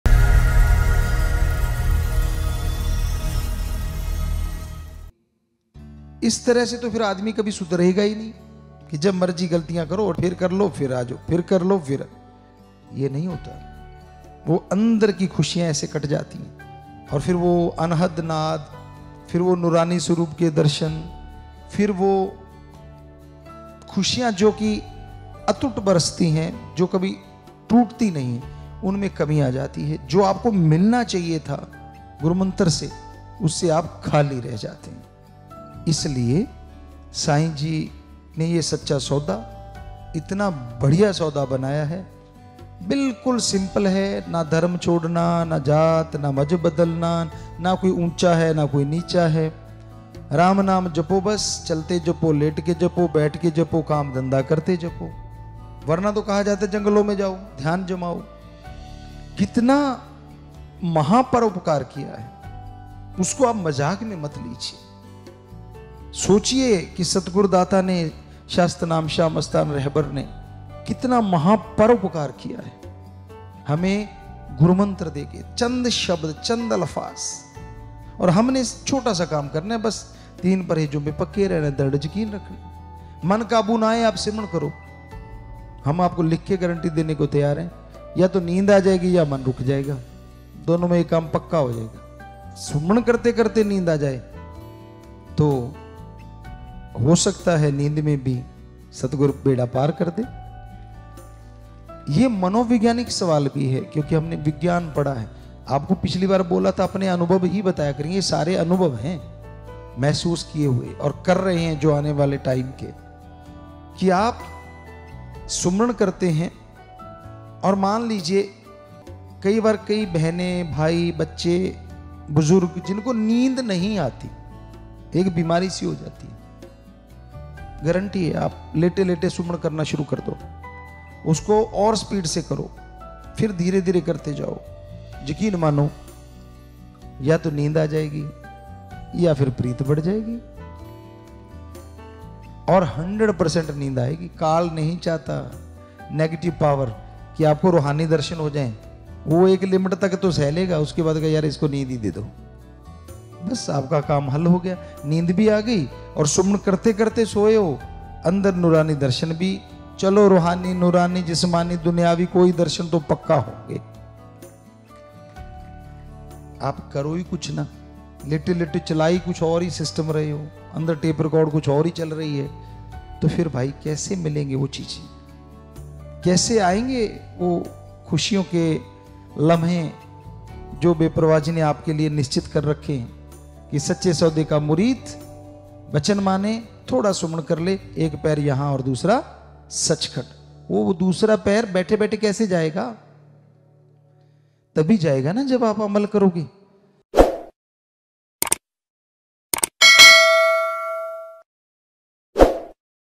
Tips for Better Sleep : ਜਿਨ੍ਹਾਂ ਨੂੰ ਚੰਗੀ ਨੀਂਦ ਨਹੀਂ ਆਉਂਦੀ, ਉਹ ਜ਼ਰੂਰ ਕਰੋ ਇਹ ਉਪਾਅ, 100% ਨੀਂਦ ਆਵੇਗੀ
Anmol Vachan : ਜਿਨ੍ਹਾਂ ਨੂੰ ਚੰਗੀ ਨੀਂਦ ਨਹੀਂ ਆਉਂਦੀ, ਉਹ ਜ਼ਰੂਰ ਕਰੋ ਇਹ ਉਪਾਅ, 100% ਨੀਂਦ ਆਵੇਗੀ || Saint MSG